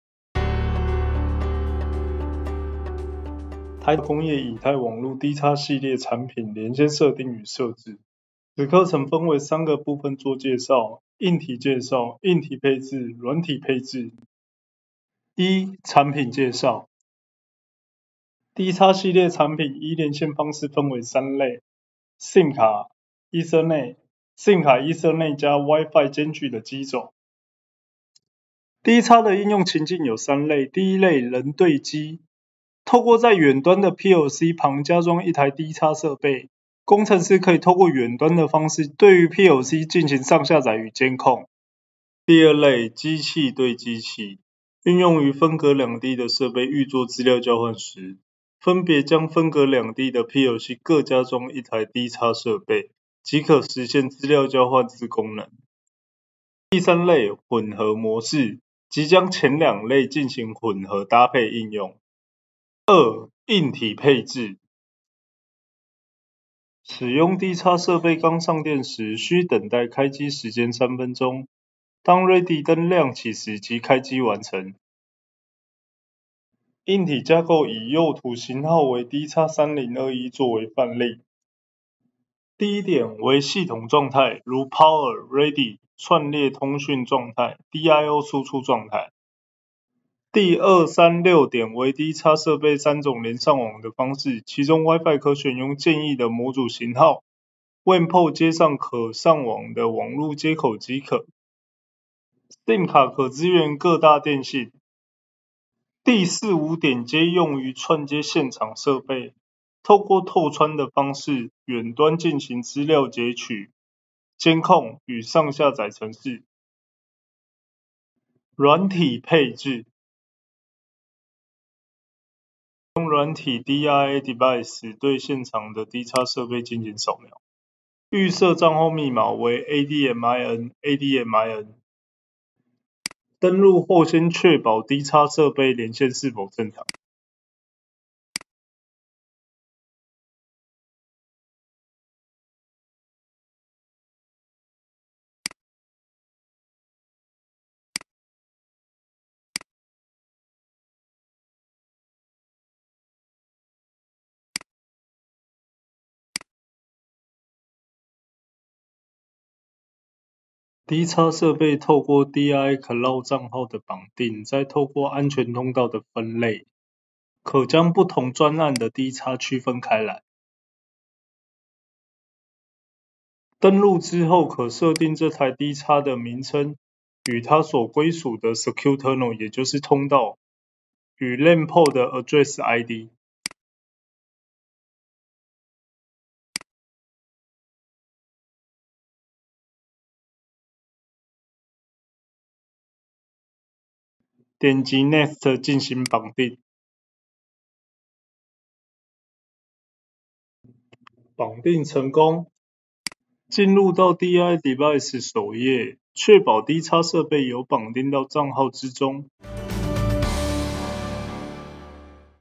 台達工業乙太網路 DX 系列 - 產品連線設定與配置
台達工業自動化 | 應用技術中心 教學
此影片介紹如何使用台達工業乙太網路 DX 系列，設定連線及配置。